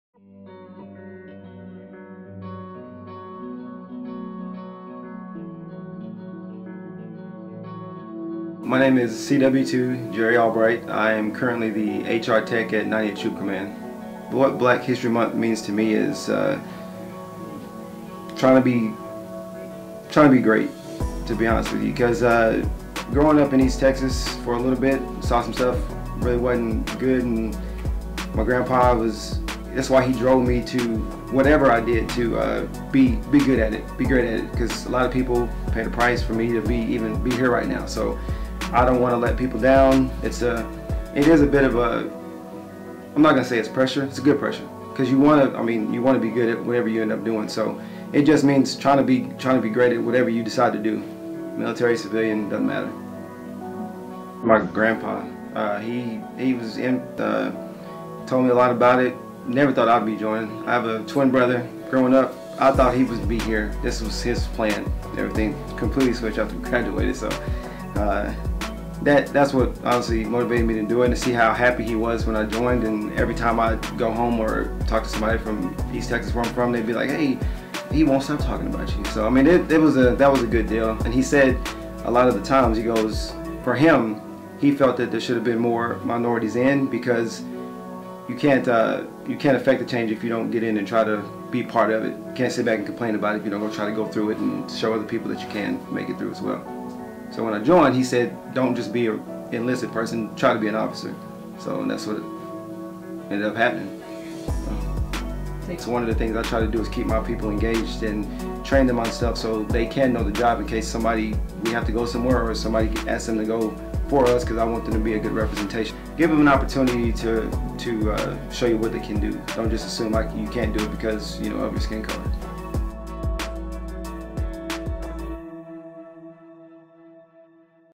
Embracing the Challenge: What Black History Month means for an Oklahoma Guardsman
“He felt there should’ve been more minorities in because you can’t affect the change if you don’t get in and try to be part of it. You can’t sit back and complain about it if you don’t try to go through it and show other people that you can make it through.”

In honor of BlackHistoryMonth, we sat down with the Oklahoma Army National Guard’s Chief Warrant Officer 2 Jerry Albright, human resource technician, 90th Troop Command, to learn what BHM means to him.

Thank you, CW2 Albright for sharing your and your family’s story, and for your service to our great state and nation!

Production Team:
Team Leader: Capt. John Brown, 145th Mobile Public Affairs Detachment
Video by Sgt. Jordan Sivayavirojna, 145th Mobile Public Affairs Detachment
Story and photos by Staff Sgt. Brigette Waltermire, 137th Special Operations Wing
Team Member: Staff Sgt. Arlenia Brown, 145th Mobile Public Affairs Detachment